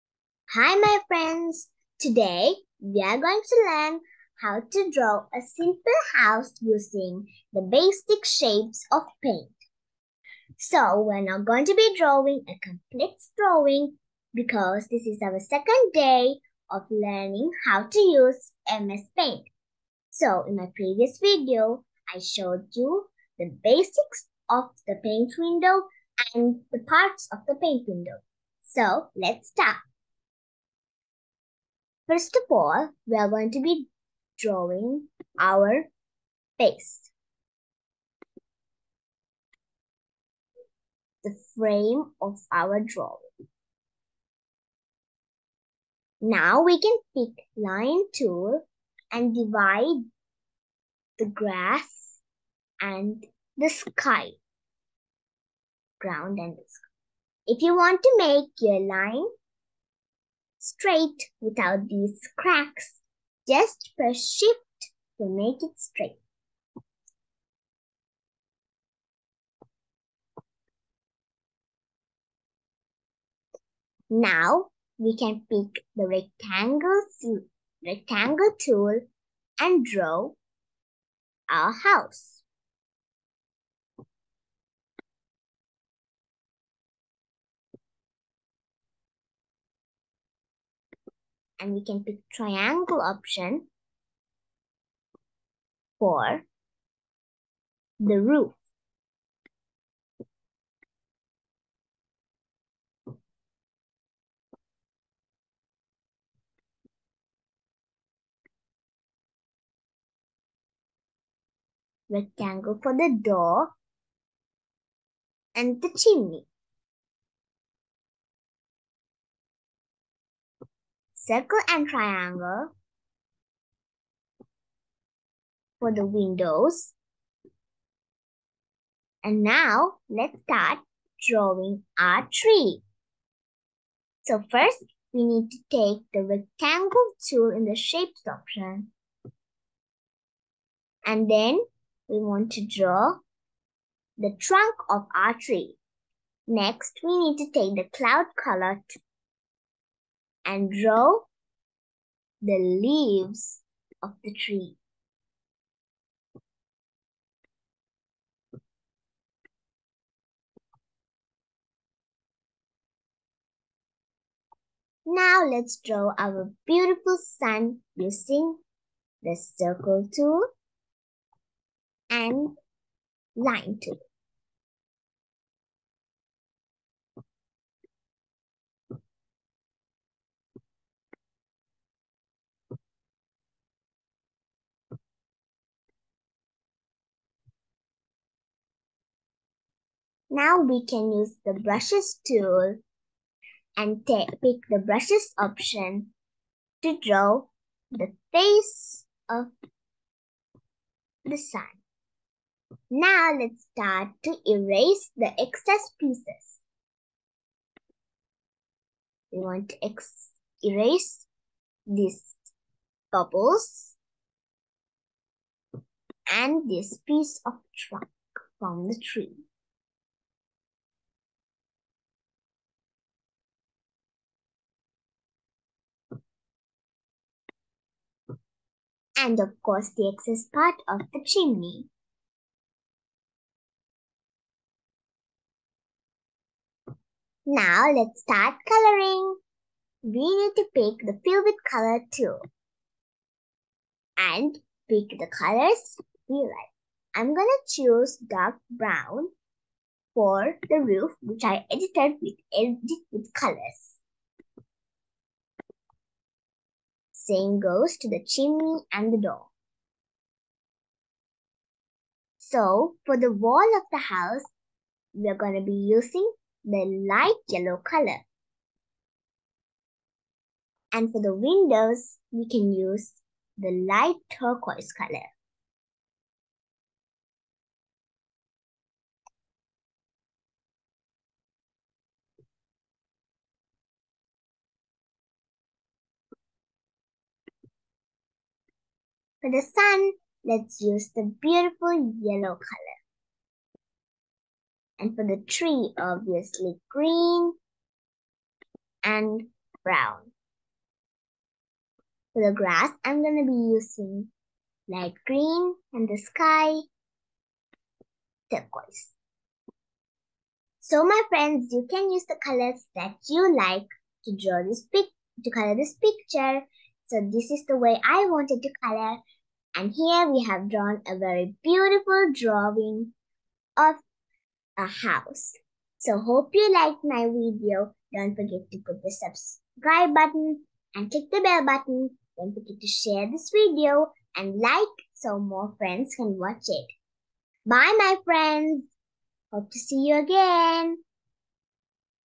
How To Draw A Simple House In MS Paint
You can learn a simple way to draw a house in MS Paint | Beginners